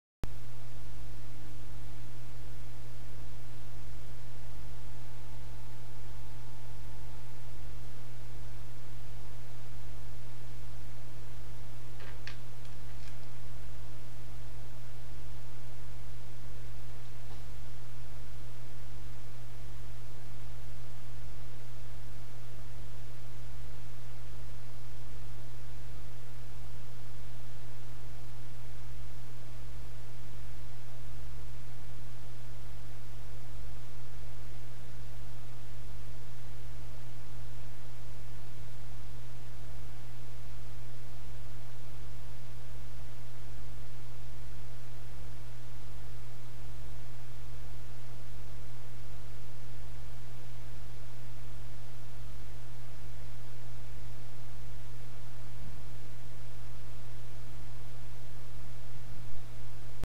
The saliva viewed with a webcam microscope
A simple hack leaded to a pretty powerful digital microscope.
This is how the saliva looks like viewed through the lenses of a cheap webcam.